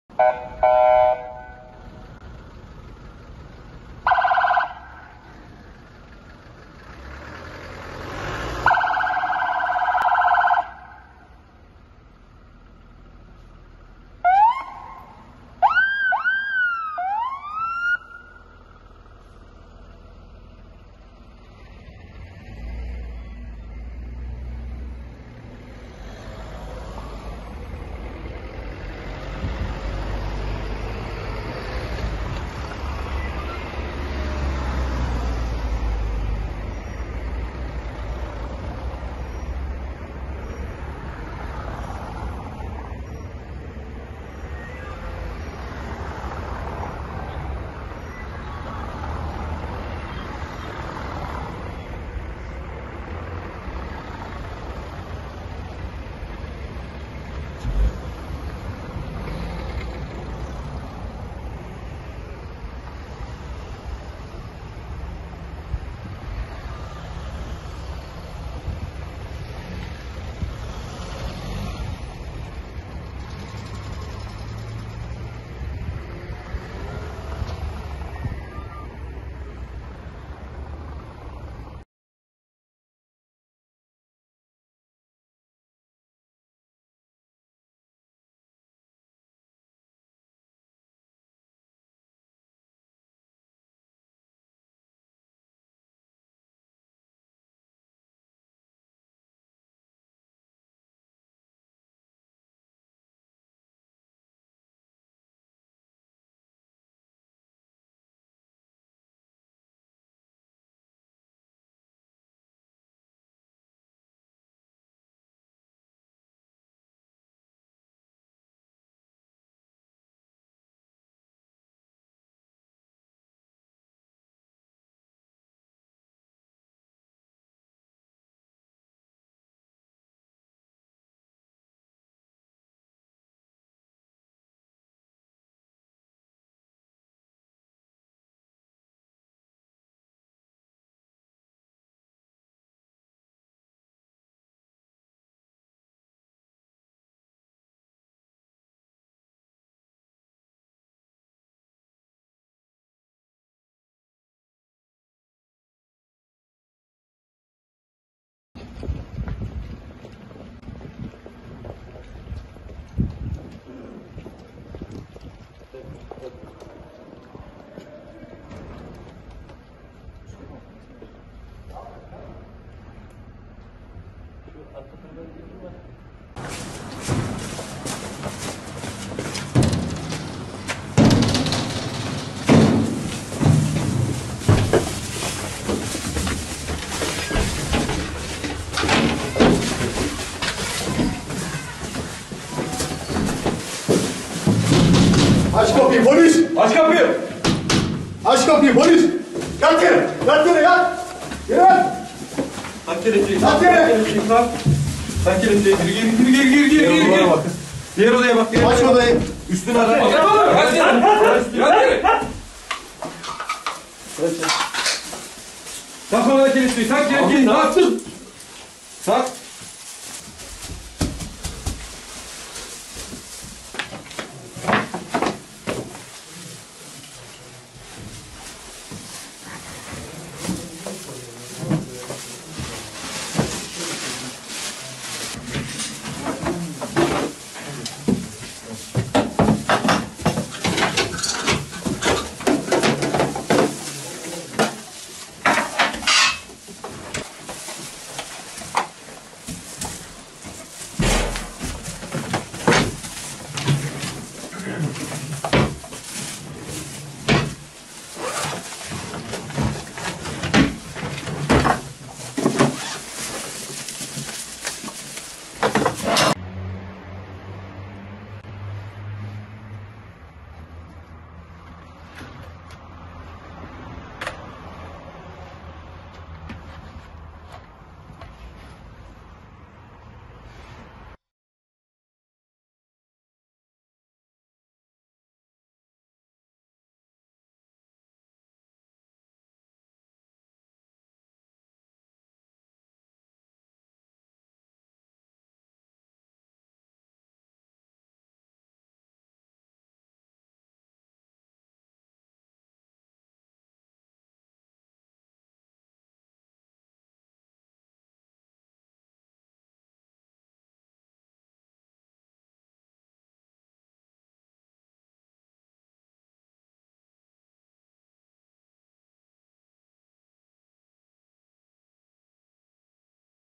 Denizli Dahil 27 İlde Eş Zamanlı 'Tırpan' Kod Adlı... - Denizli Haberleri - HABERDENİZLİ.COM
Emniyet Genel Müdürlüğü Kaçakçılık ve Organize Suçlarla Mücadele Daire Başkanlığı (KOM) ekiplerince, mafya tipi organize suç örgütleri ile bu örgütlere silah temin eden suç gruplarına yönelik; 27 ilde eş zamanlı 'Tırpan' kod adlı operasyon başlatıldı. Operasyon, 13 organize suç örgütü ile bu örgütlere silah temin eden 28 suç grubuna yönelik gerçekleştiriliyor. Haber Denizli Youtube Kanalı